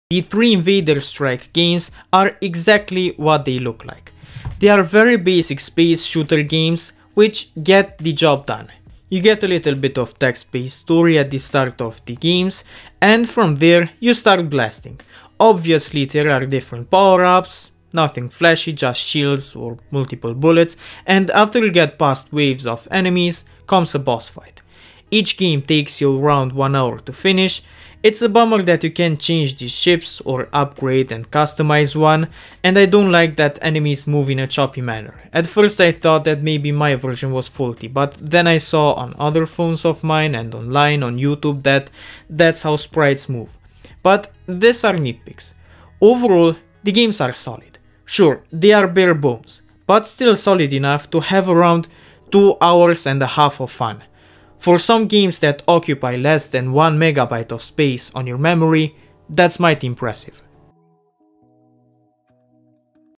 All Invaders Strike Games for Java Review
Here is a list of all Invaders Strike Games I've found for Java:
1. Invaders Strike
2. Invaders Strike 2
3. Invaders Strike 3

The 3 Invaders Strike Games are exactly what they look like. They are very basic space shooter games, which get the job done. You get a little bit of text based story at the start of the games, and from there, you start blasting. Obviously there are different power ups, nothing flashy, just shields, or multiple bullets, and after you get past waves of enemies comes a boss fight. Each game takes around 1 hour to finish. It’s a bummer that you can’t change ships or upgrade and customize one, and I don’t like that enemies move in a choppy manner. At first I thought that maybe my version was faulty, but then I saw on other phones of mine, and online on youtube that that’s how sprites move.  But these are nitpicks. Overall, the games are solid. Sure, they are bare bones, but still solid enough to have around 2 hours and a half of fun. For some games that occupy less that 1 mb of space on your memory, that’s mighty impressive.